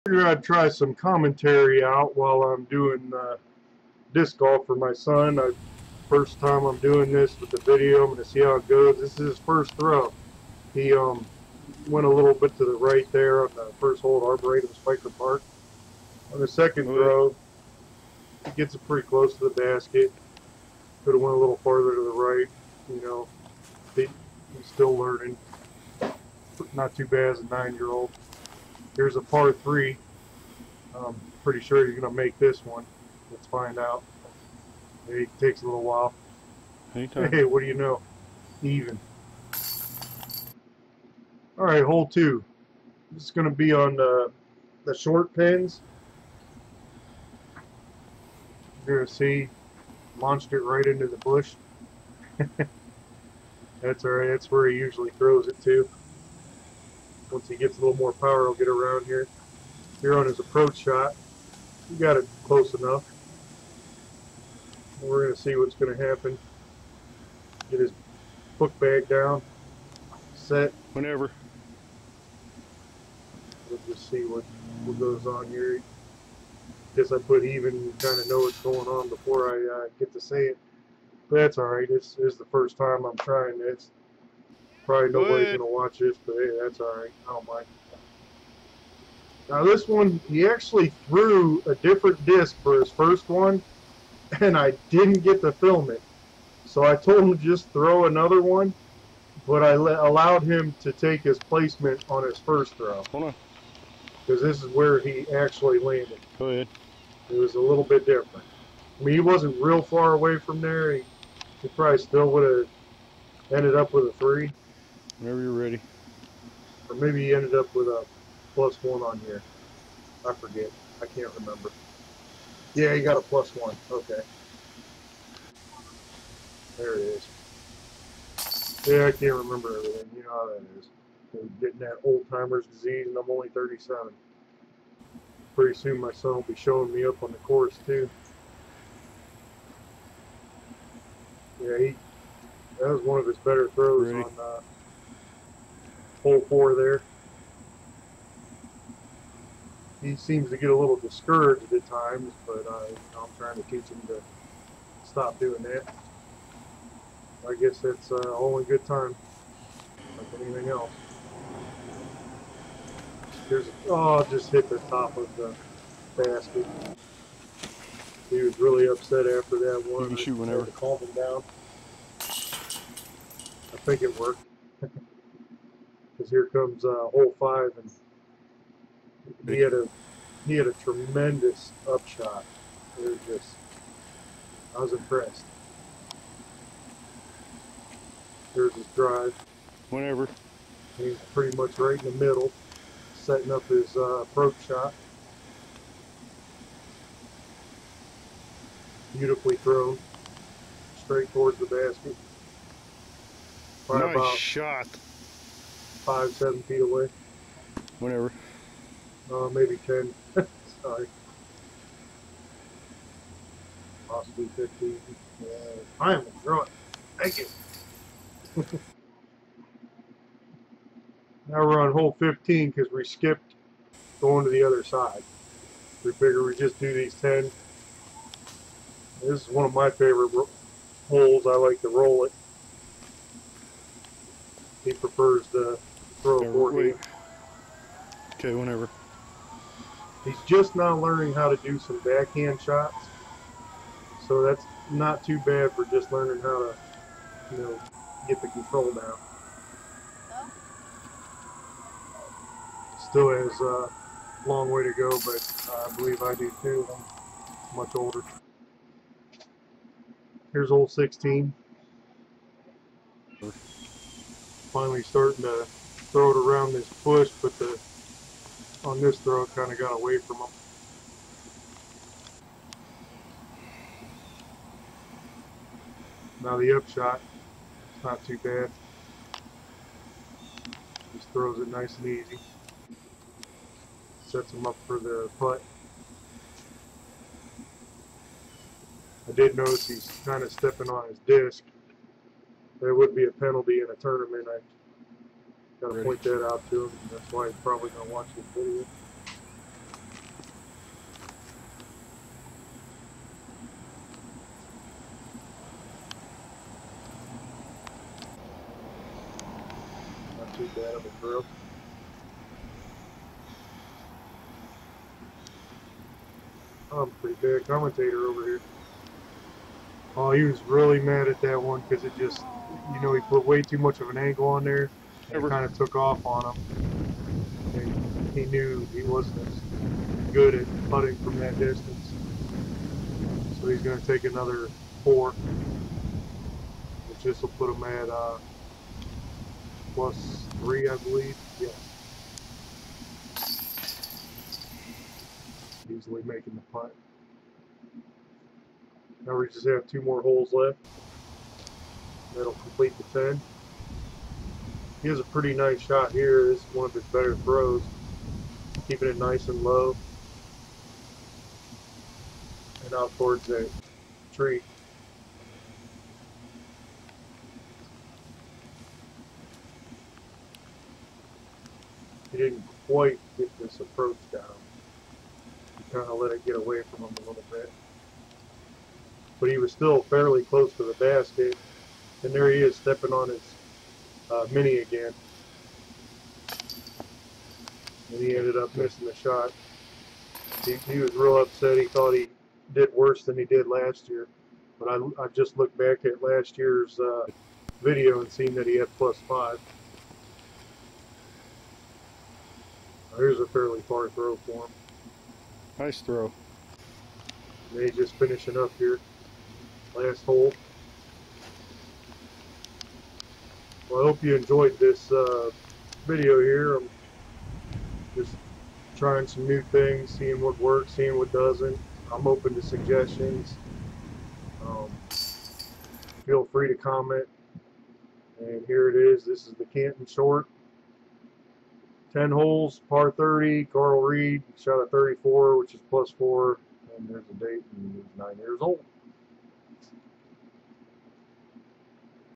Arboretum Spiker Park Karl Reed 9 year old disc golfer 9 8 16
Karl Reed at 9 years old shot a +4 at Canton Short course in disc golf